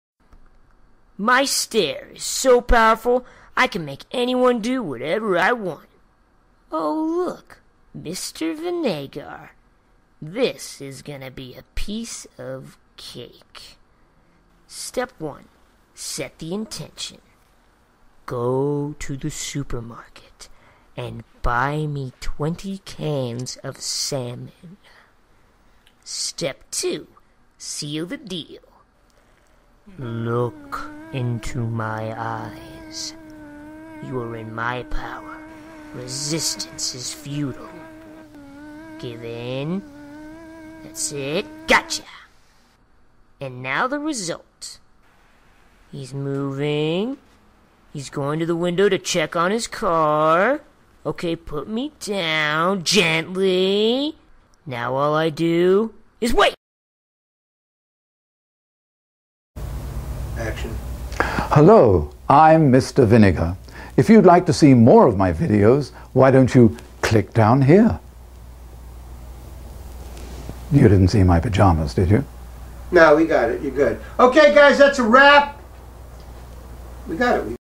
The Stare.
Jonesy demonstrates his hypnotic power over MR VINÉGAR.